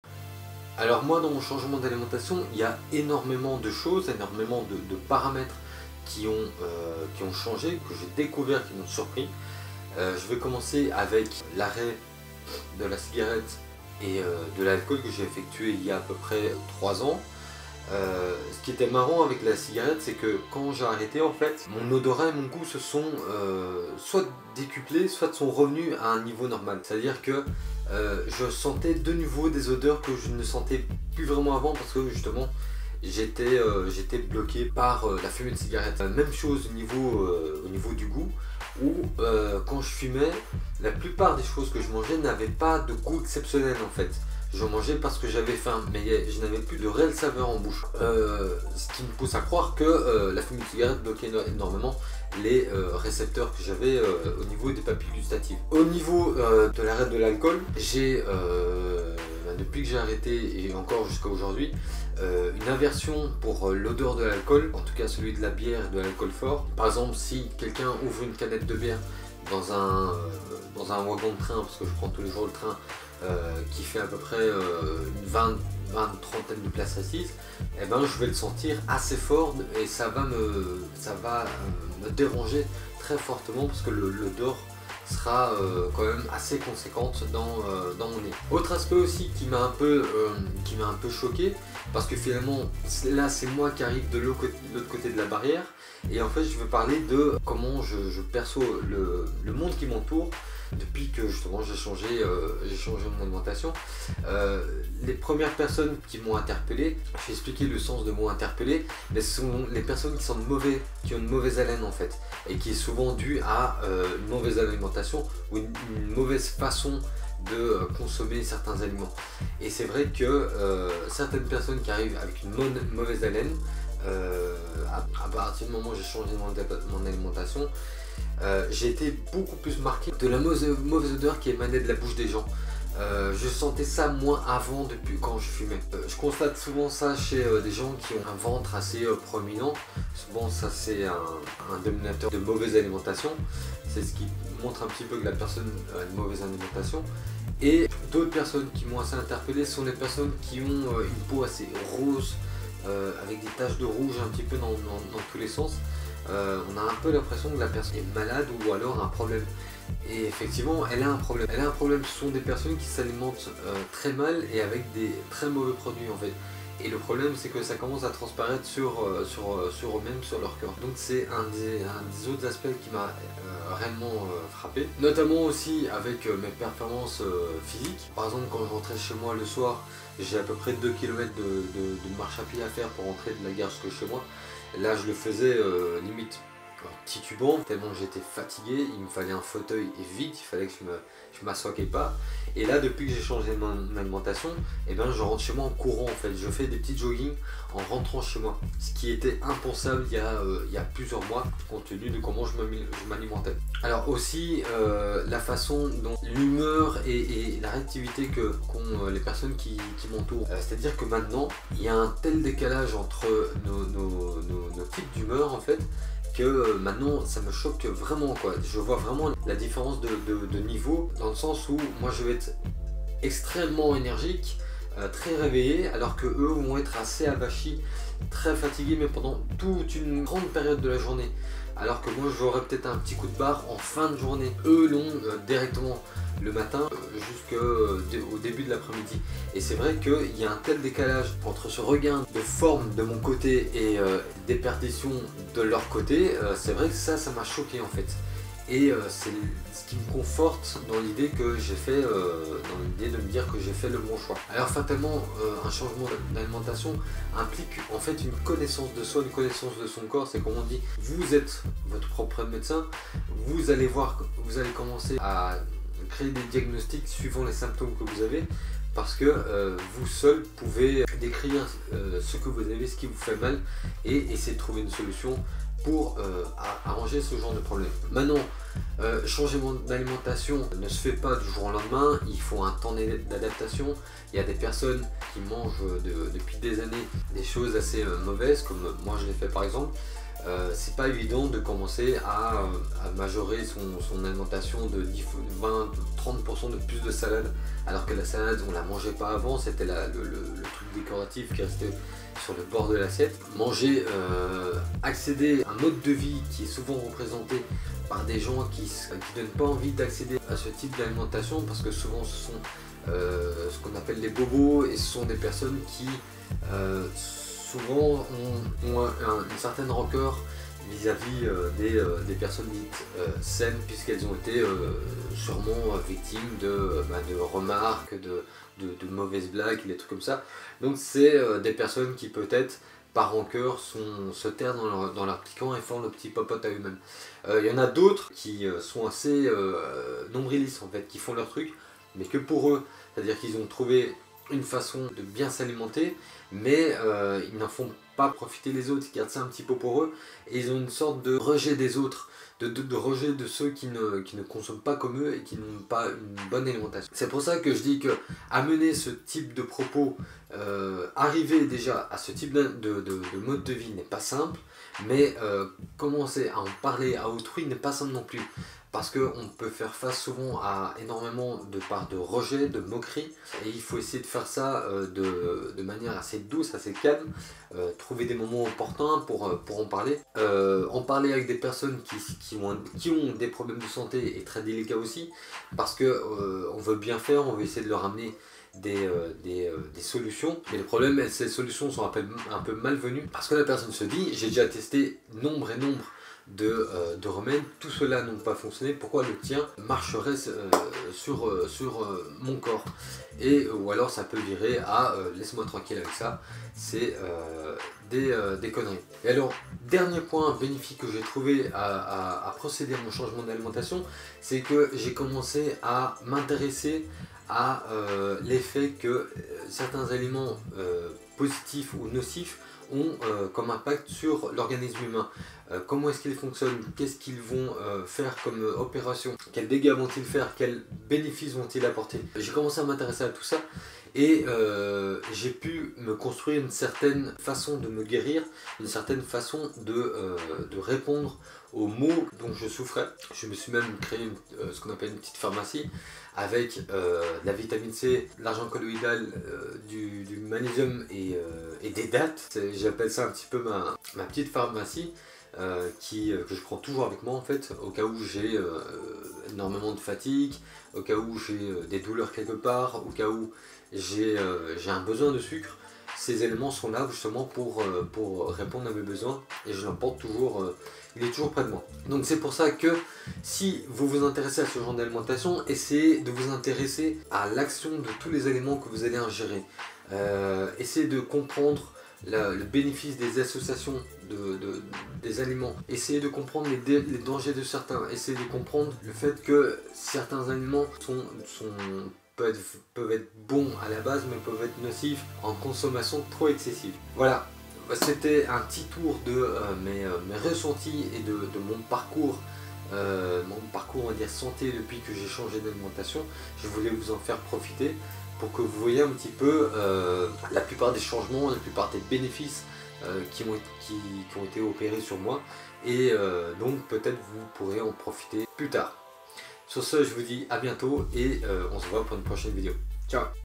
Alimentation : ce qui a changé (pour ma santé)
Voici un recapitulatif de ce qui a changé pour moi depuis mon alimentation alternative